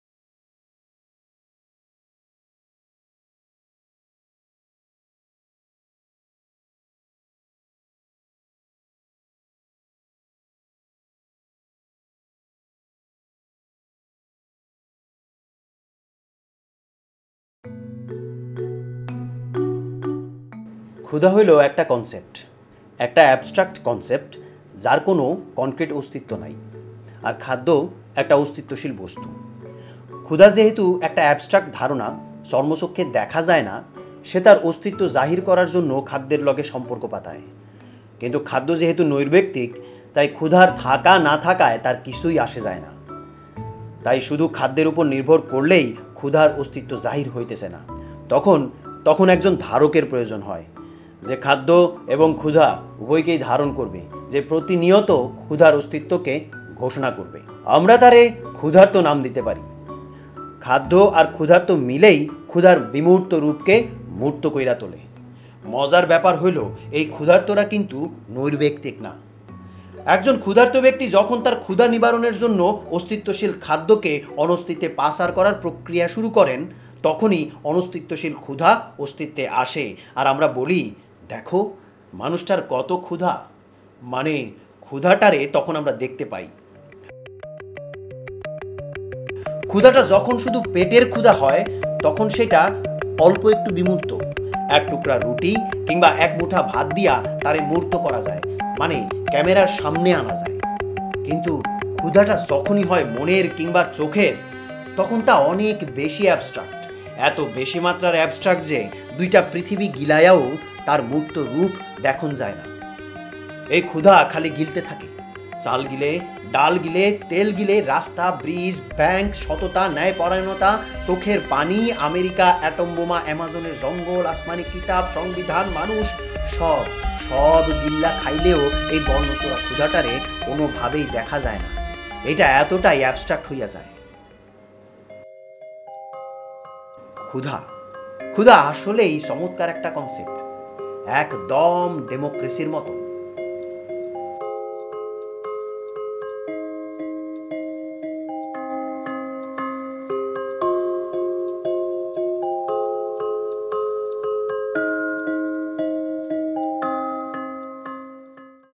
Hungriocracy
It is very interesting when you juxtaposed images, sound and text from different origin, they are creating a different and completely new meaning together. I am doing this experiment in many ways. For this current project, Hungriocracy, I followed Jean-Luc Godard's structure of his 'Histoire(s) du Cinema'. I found this 'Photomontage with narration' style is very much unique and effective way to create new meaning in the images.
Here is the English translation of the text that I recited in the film. Yes, it is, indeed, a film to me.

The text:
Hunger is a concept, an abstract concept that does not have any concrete existence. On the other hand food is an existential object. Since hunger is an abstract concept, invisible to the naked eye, it forms a relationship with food to assert its existence. But since food is objective, it doesn't matter to it if the hunger exists or not. So the existence of hunger is not only manifested by relying on food, then there is a need for a container, that will contain both food and hunger, that will constantly declare the existence of hunger. We can name him hungry. The combination of food and hunger embodies the abstract form of hunger. The interesting thing is that these hungry people are not objective. When a hungry person starts the process of sending the existent food into non-existence to satisfy his hunger, non-existent hunger comes into existence and we say, look, how hungry the man is! It means, we can see the hunger then.

When hunger is only the hunger of the stomach, it is a little abstract. With a piece of bread or a handful of rice, it can be embodied, I mean, it can be brought in front of the camera. But whenever hunger is the hunger of mind or hunger of eyes, it is so abstract that its form cannot be seen even after swallowing two worlds. This hunger just keeps swallowing. It swallows rice, it swallows pulses, it swallows oil, roads, bridges, banks, honesty, justice, tears, America, the atom bombs, the jungle of the Amazon, the heavenly books, the constitution, the human being -- even after swallowing everything, this deceptive hunger cannot be seen in any way. It becomes so abstract.

Hunger is really a great concept, just like democracy.

Hungriocrecy
Directed by Masudur Rahman
Photography: Umme Saima Sorna
Video Editing: Masudur Rahman